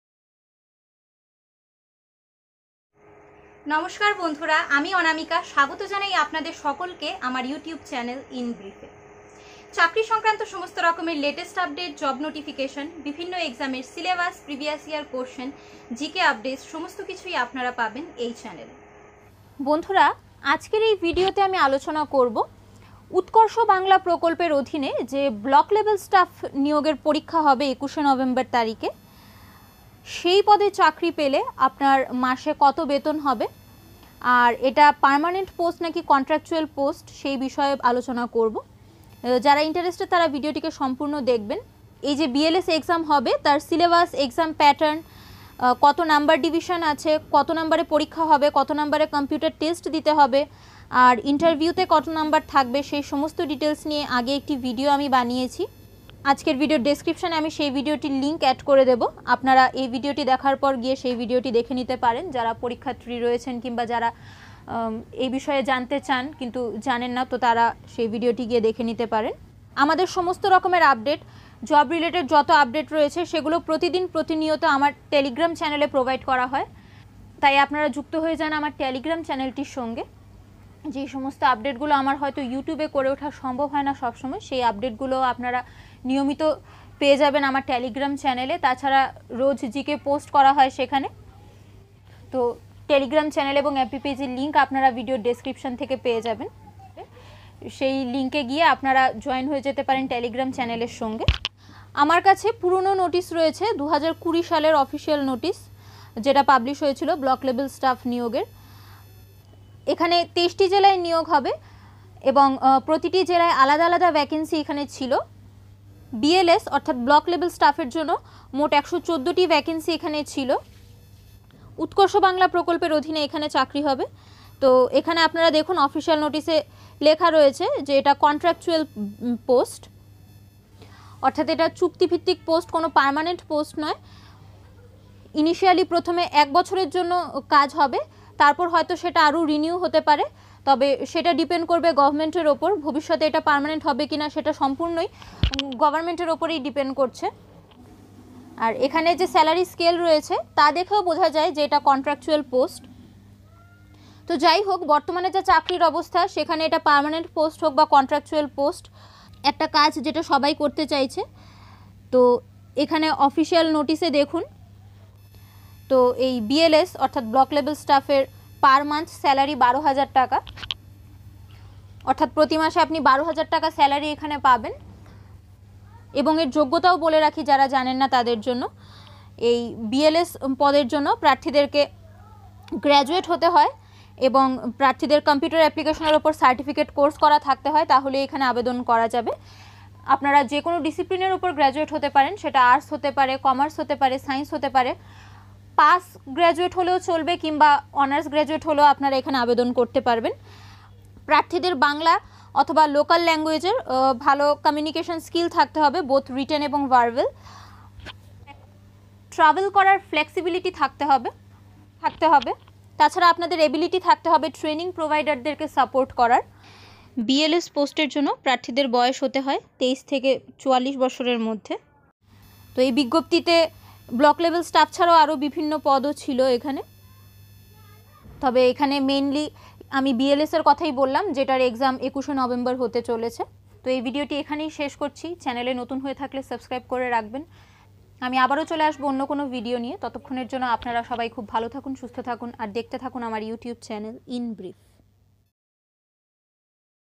উৎকর্ষ বাংলা BLS পদের স্যালারি কত জেনে নিন | Block Level Staff Salary Per Month
This video contains information about recent west bengal bls exan  2021 eligibility , age limit & salary etc. So, watch the video till end.
Here you will get all the details of Central & State govt. various  post recruitment 2020 such as eligibility, vacant post, important dates,cut off etc.

Topic Cover :

Salary for Block level staff
উৎকর্ষ বাংলা BLS স্যালারি
ব্লক লেভেল স্টাফ স্যালারি
West Bengal Block Level Staff Salary 2021
BLS Salary 2021
West Bengal BLS Salary 2021